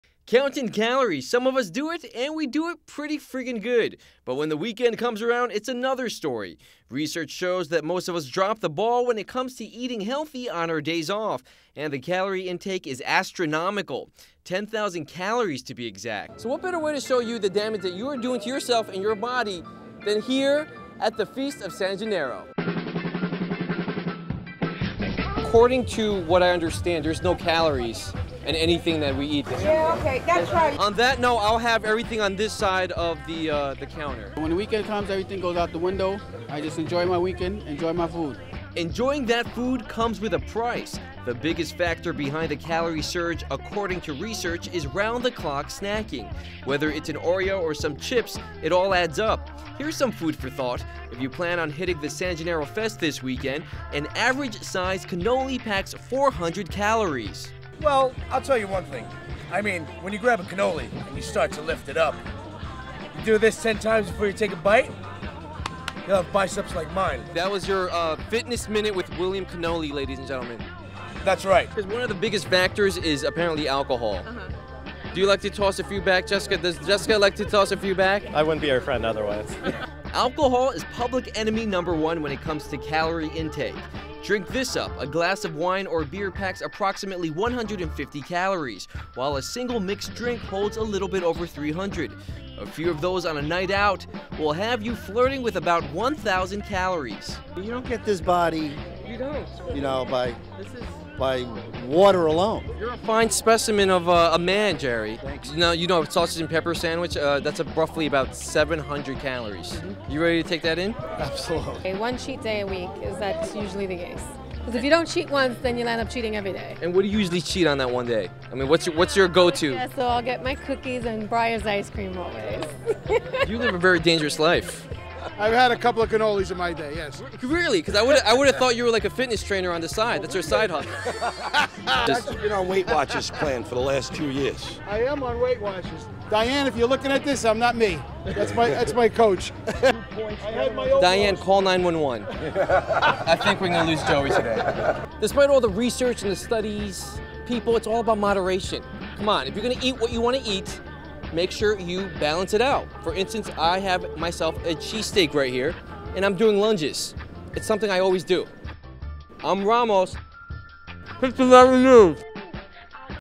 Why the weekends are ruining your chances of losing weight
Some of us count calories and for the most part-- we do a pretty good job of it. However, when the weekend rolls by, we all of a sudden forget how to do math.

Research shows that most of us drop the ball when it comes to eating healthy on our days of and the calorie intake is astronomical -- 10,000 calories to be exact.